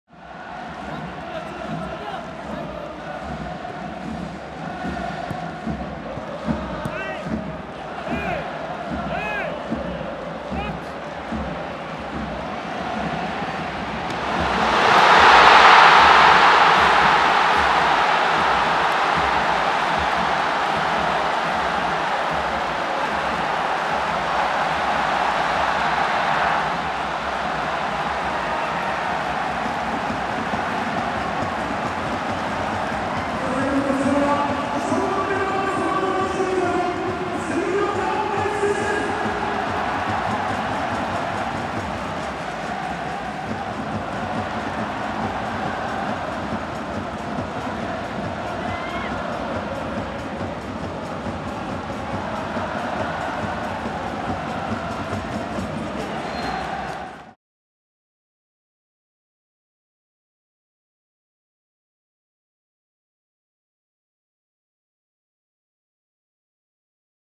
【公式】ゴール動画：杉岡 大暉（湘南）36分 湘南ベルマーレvs横浜Ｆ・マリノス ＪリーグYBCルヴァンカップ 決勝 2018/10/27
前線でこぼれ球を拾った杉岡 大暉（湘南）がミドルレンジから左足を豪快に振り抜くと、強烈なシュートが相手GKの手を弾きながらもゴールに吸い込まれて先制点を奪取！
2018年10月27日（土）に行われたＪリーグYBCルヴァンカップ 決勝 湘南vs横浜FMのゴール動画です。